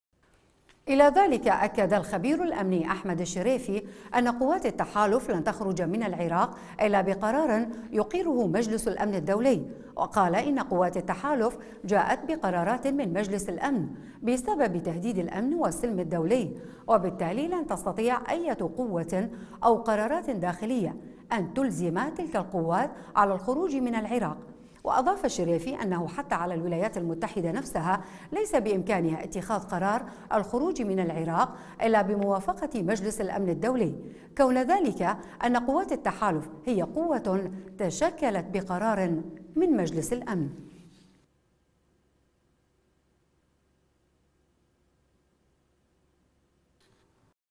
الشريفي : مستقبل التحالف الدولي يقرره مجلس الأمن
العراق وامريكيا

الى ذلك , أكد الخبير الأمني أحمد الشريفي ، أن قوات التحالف لن تخرج من العراق إلا بقرار يقره مجلس الأمن الدولي. وقال إن قوات التحالف جاءت بقرارات من مجلس الأمن بسبب تهديد الأمن والسلم الدولي وبالتالي لن تستطيع أية قوة أو قرارات داخلية ان تلزم تلك القوات على الخروج من العراق .وأضاف الشريفي أن حتى الولايات المتحدة نفسها ليس بإمكانها اتخاذ قرار الخروج من العراق إلا بموافقة مجلس الأمن الدولي كون أن قوات التحالف هي قوة تشكلت بقرار من مجلس الامن .